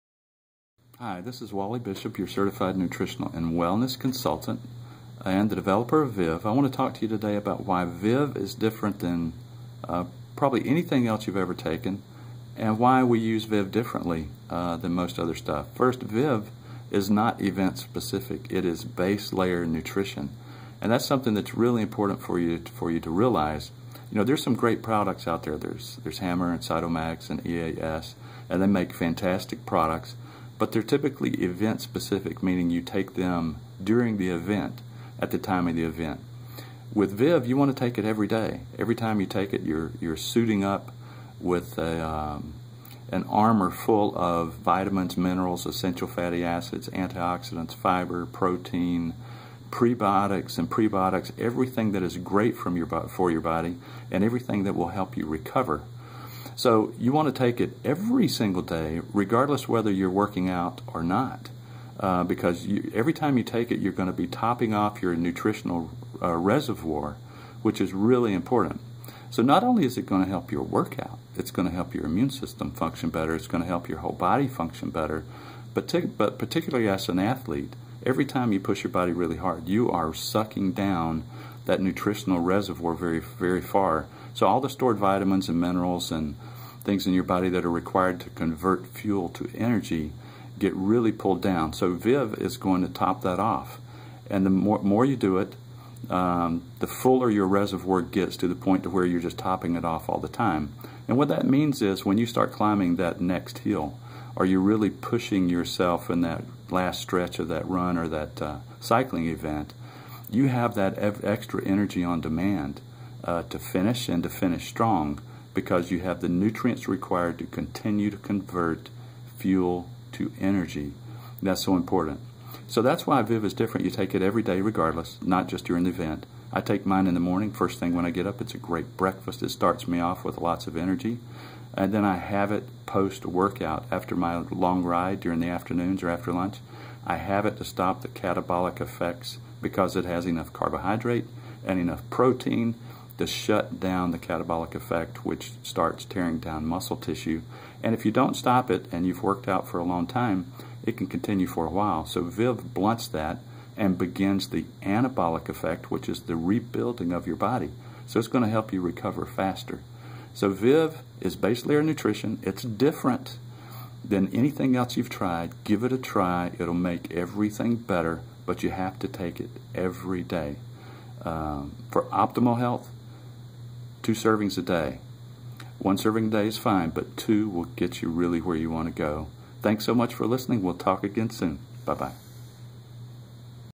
Vive Shake - Event Specific Sports Drinks vs. Vive Shake
Wally Bishop CNC discusses the difference between base layer nutrition in Vive Shake vs. event specific sports drinks.